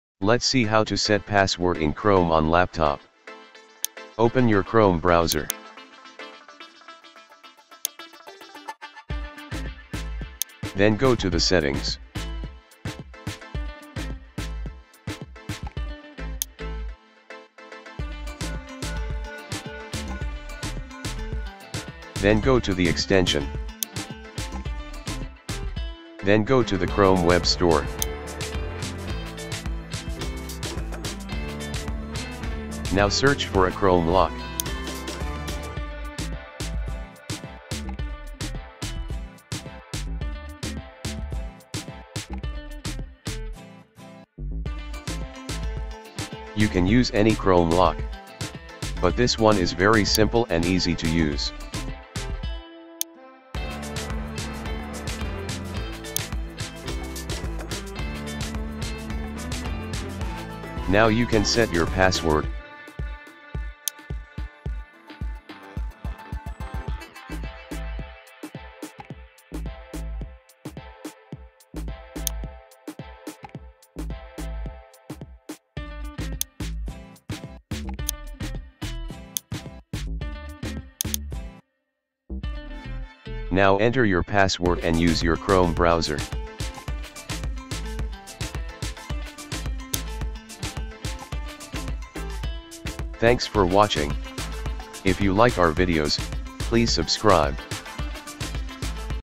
How to put password on chrome in laptop
How to set password for chrome in pc. How to lock google chrome browser with password